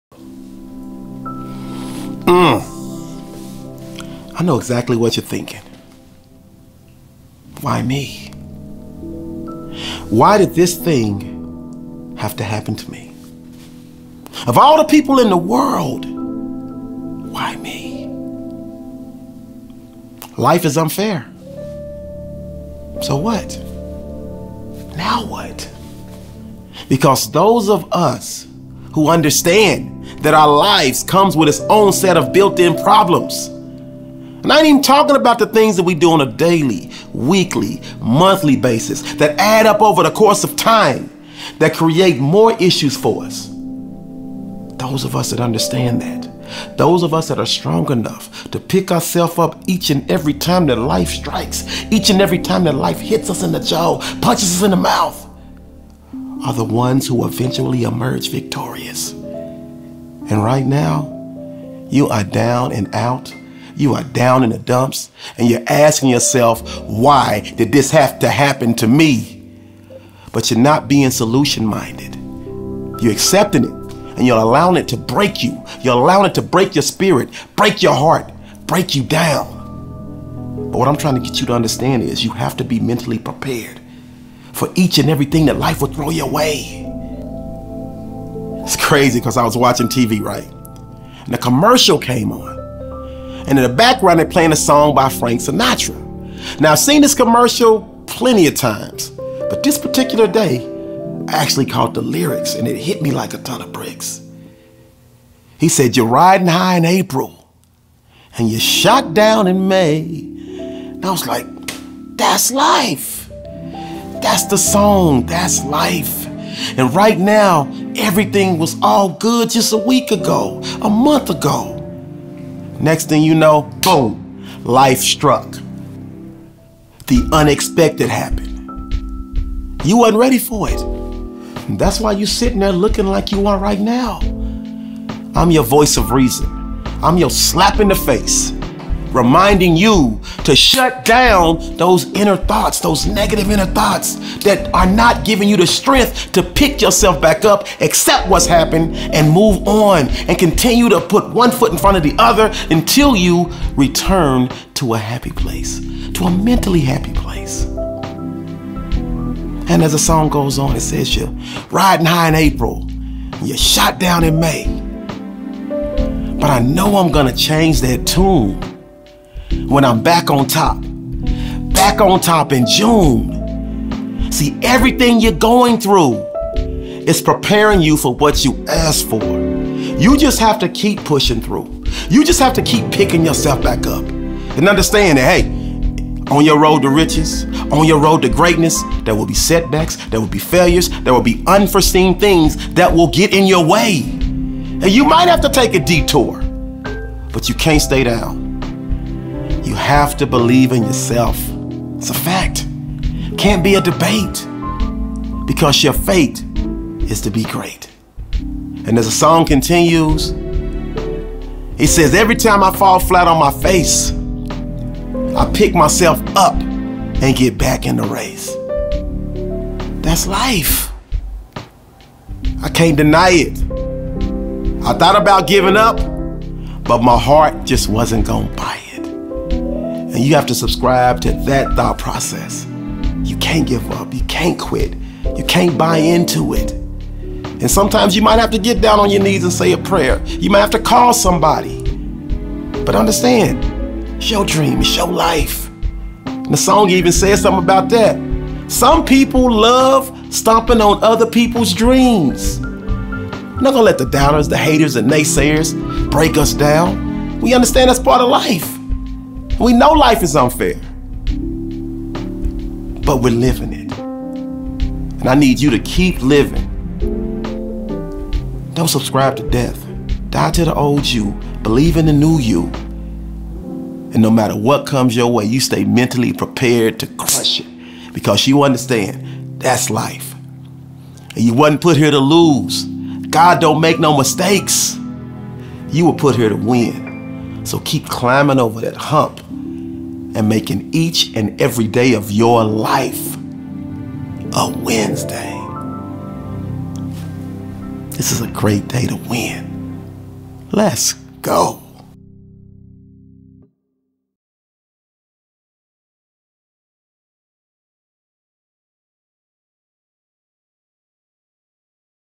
Life Is So Unfair (Now What) | Daily Motivation Dose | Freddy Fri Motivation
In this video, I want to remind you that life is so unfair, but that is never a reason to give up on the greatness you are destined to achieve! My name is Freddy Fri and I welcome you to Freddy Fri Motivation!

I know exactly what you are thinking, why me, and if you are this episode is designed for you to remind you that life is so unfair, but that is never a reason to give up on the greatness you are destined to achieve!

You are wondering why it happens to you, out of all the people in the world, why you?

Well, you need to remember that life is unfair but so what, you need to understand that everyone's life comes with a set of built-in problems, but it's those of us that are able to keep going, keep picking ourselves up that reach success and become victorious.

You need to learn to be more solution-minded, your just accepting it and you are allowing it to break you, allowing it to break your spirit, break your heartbreak you down, but you need to learn to be mentally prepared for each and everything that life will throw your way.

So I want you to simply just shut down those negative thoughts and not to let them surface.